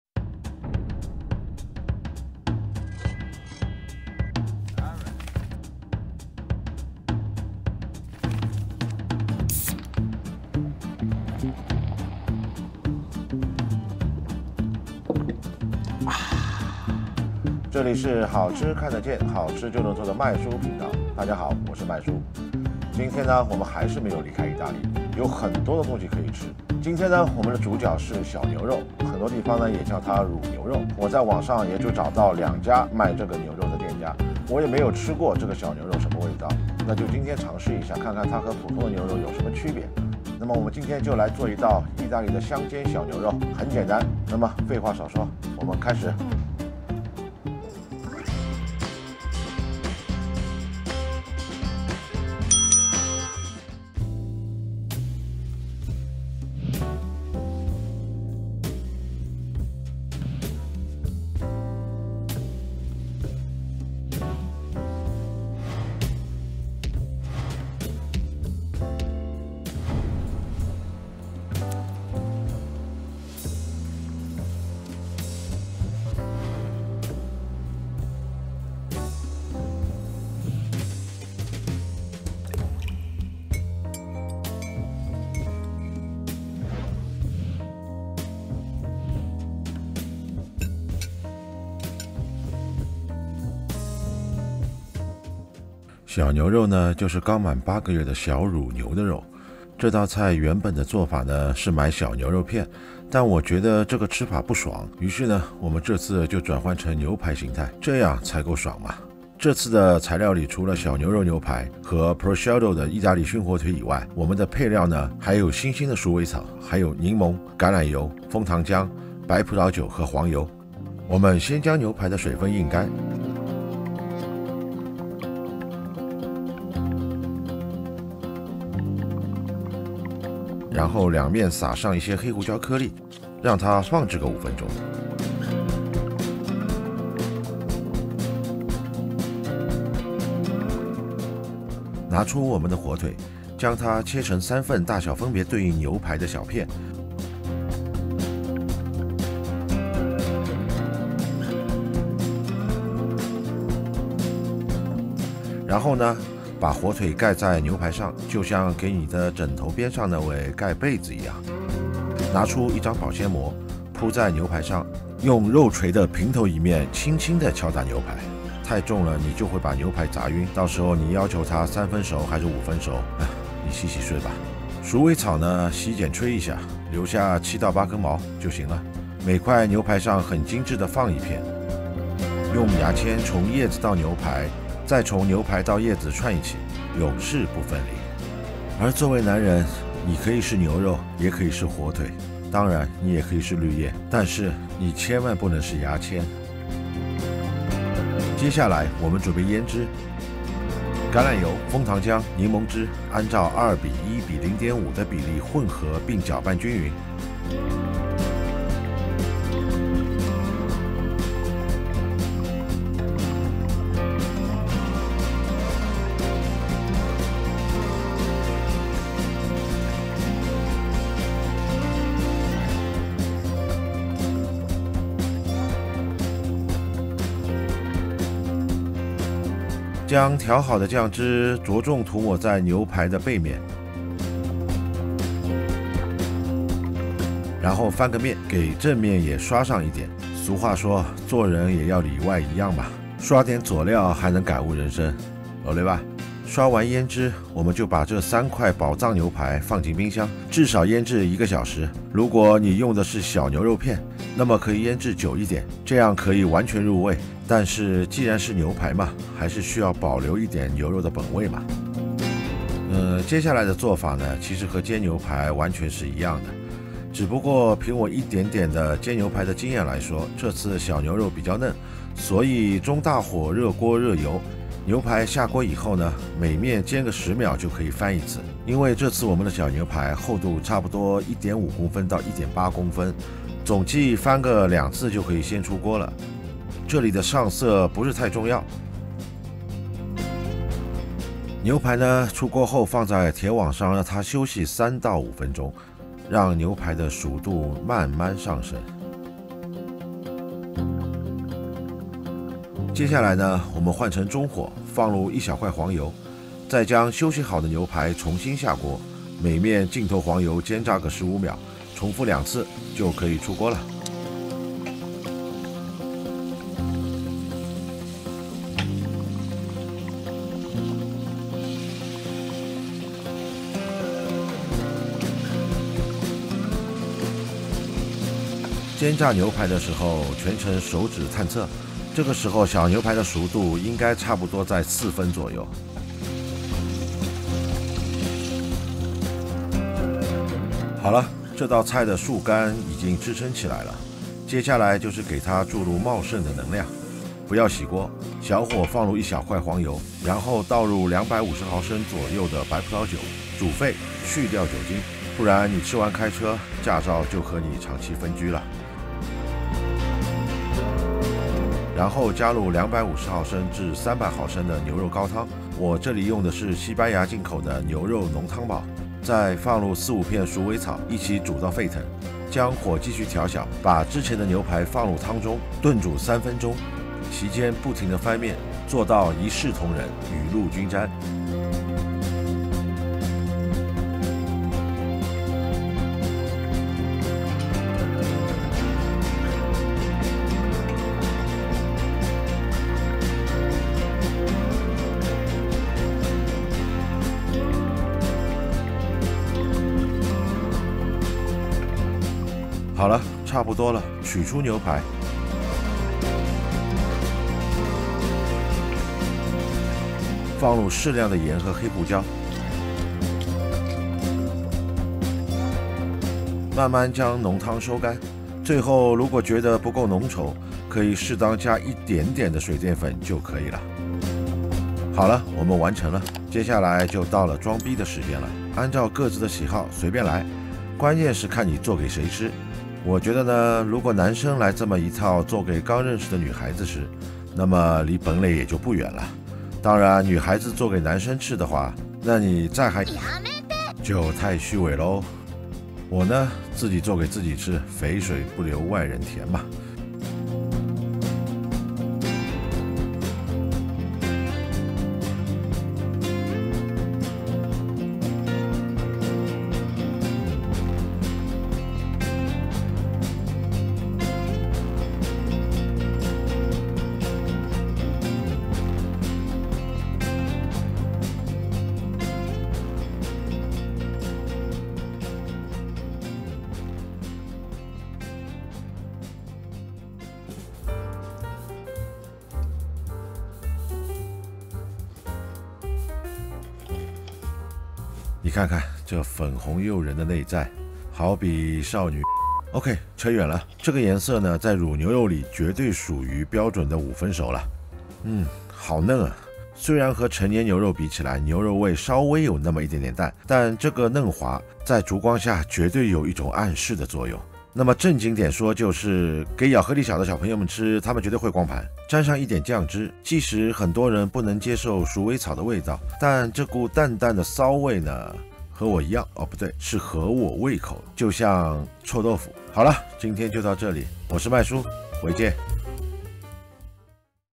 经典意大利香煎小牛肉，这次有点不一样，吃起来更爽更优雅！Cooking the Italian fried veal like me. tastes better and more elegant!
你还可以来这看到我：
意大利经典菜，香煎小牛肉！一如既往，我们稍稍做了改动，比起传统吃法应该更美味，更优雅！
小牛肉，又叫乳牛肉，即只有8个月的小牛的肉，肉质很嫩，非常时候需要补充牛肉营养，又怕咬不动的小朋友。也适合喜欢嫩口的女孩子，关键这样做颜值高，姿势优雅，关键味道个人觉得应该更好。
鼠尾草作为意大利的传统香料，在这款美食里起到了关键作用，好了，大家看视频吧